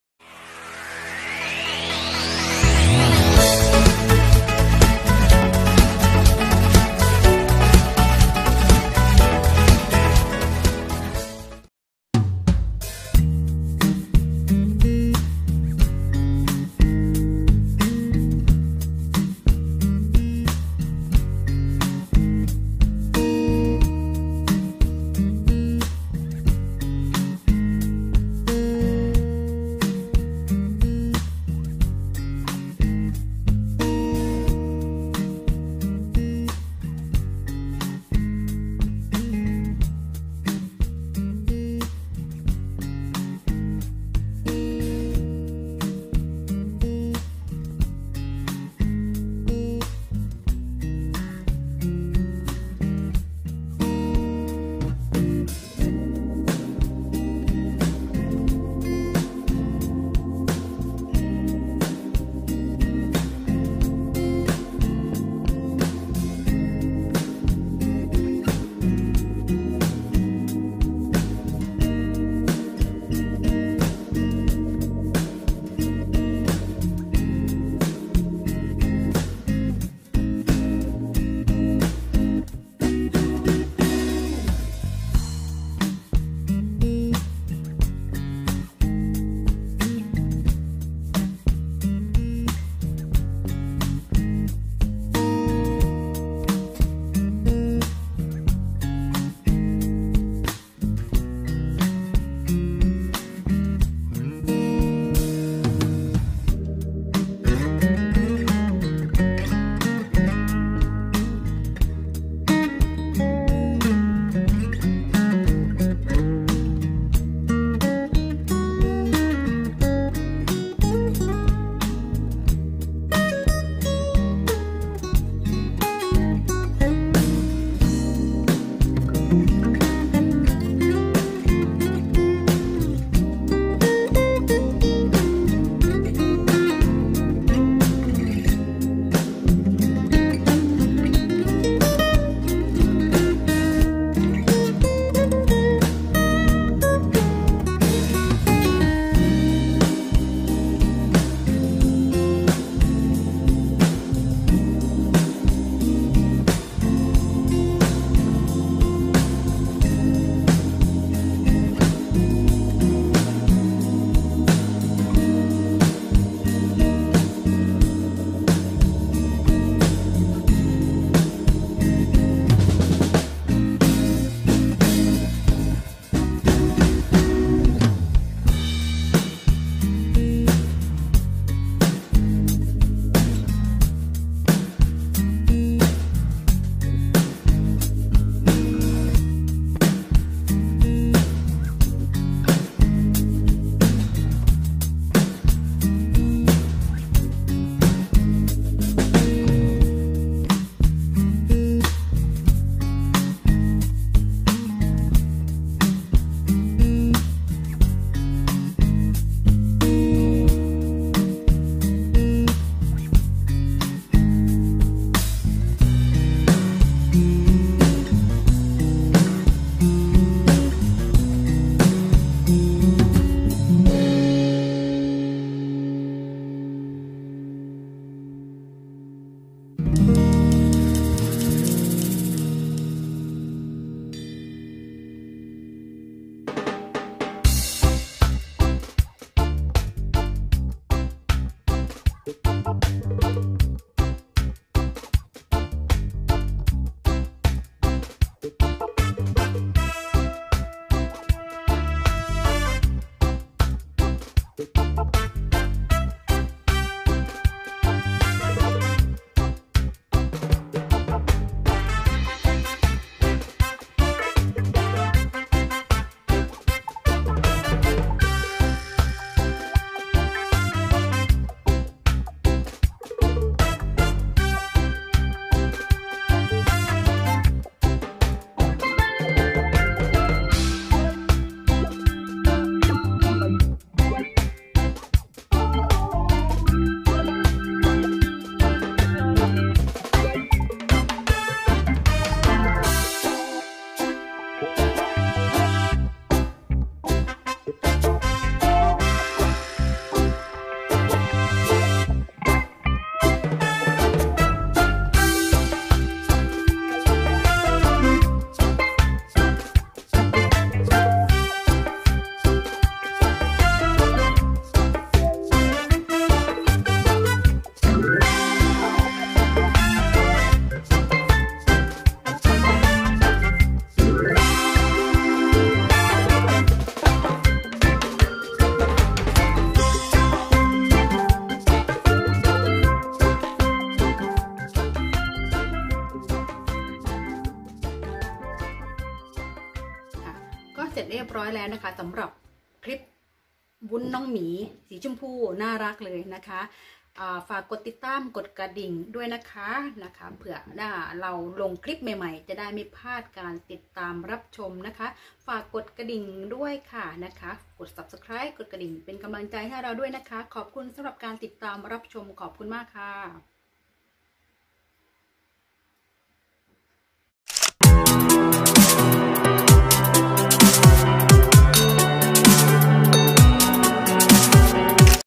วิธีทำ วุ้น น้องหมี ทำกินก็ได้ ทำขาย ก็ดี
วิธีทำ วุ้น น้องหมี ทำกินก็ได้ ทำขาย ก็ดี @phat-homemade

ว่างๆ มาทำวุ้นกันคะ
กะทิ  1 ถ้วย หรือ 250 ml.
น้ำเปล่า 1 ถ้วย หรือ 250 ml.
น้ำตาลทราย 1/2 ถ้วย
ผงวุ้น 1/2  ช้อนโต๊ะ
เกลือ
แป้งท้าว
สีผสมอาหารสีต่างๆตามชอบ

ส่วนขั้นตอนและวิธีทำ รับชมได้ในคลิปเลยคะ

| พักสักครู่ take it easy |  ขอขอบคุณทุกกำลังใจ และทุกการติดตามรับชมนะคะ

เรายังมีสูตรอาหาร เมนูอาหาร อื่นๆอีกมากมาย

วิธีทำ เห็ดเข็มทอง เบคอน

พักสักครู่ take it easy